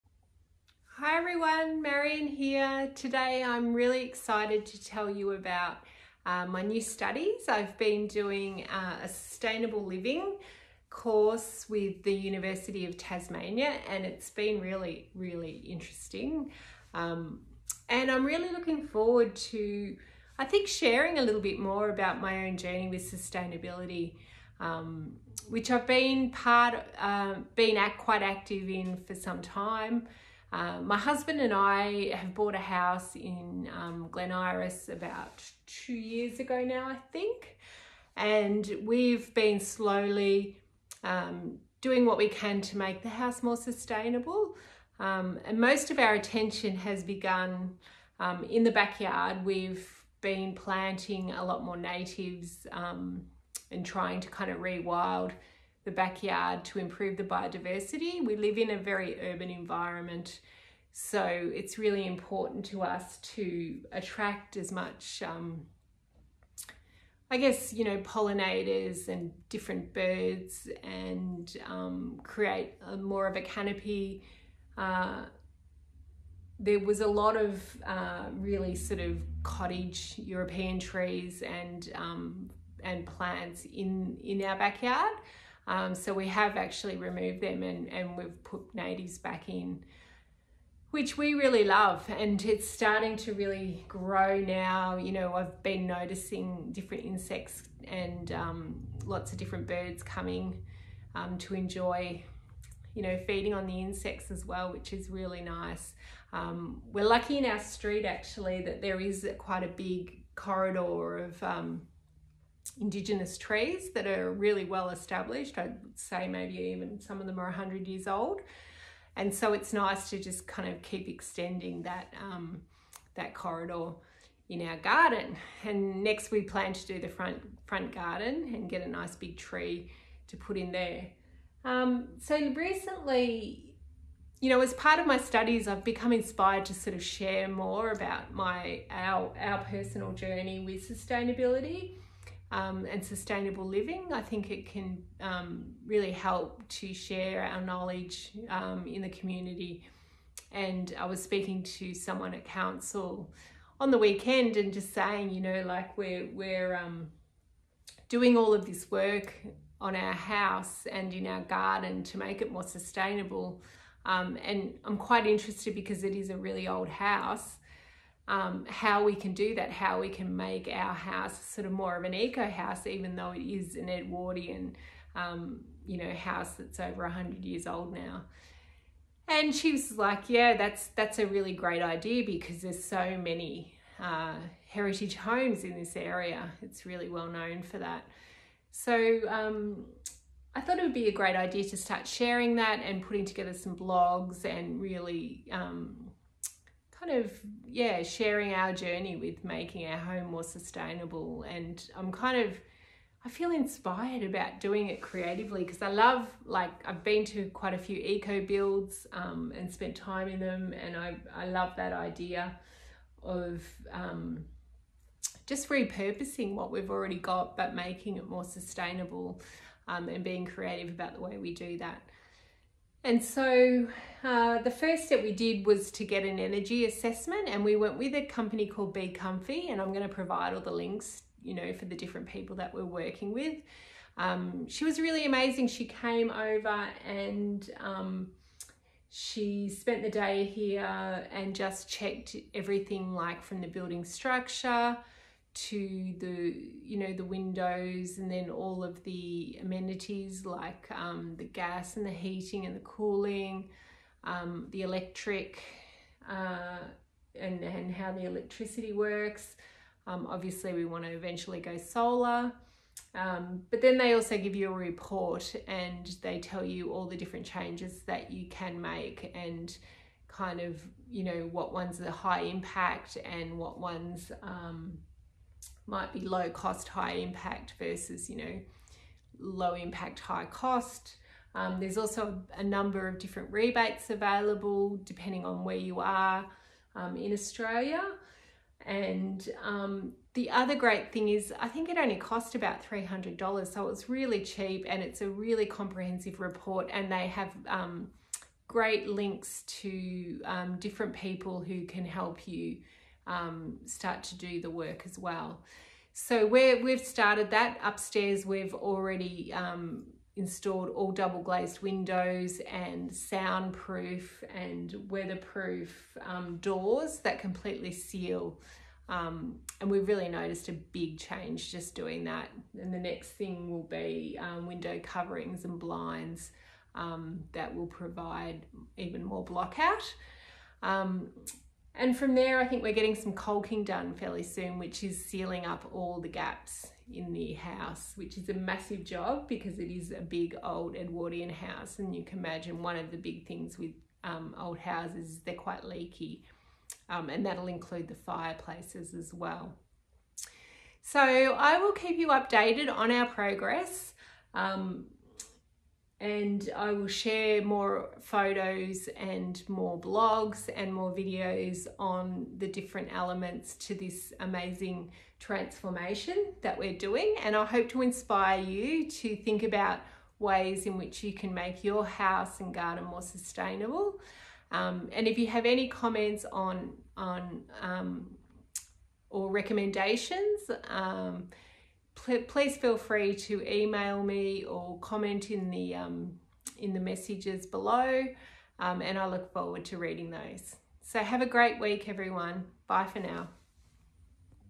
Sustainable Living Series: Making the house and garden more sustainable
This year, I embarked on a Sustainable Living course at the University of Tasmania, and it has been an incredibly enjoyable experience. In conjunction with my work in ecotherapy, I am actively seeking ways to integrate sustainable practices into both my personal life and professional endeavours. In this first video I discuss ways I am making my home, studio and garden more sustainable. Click the link to find out more and access the links mentioned in the video.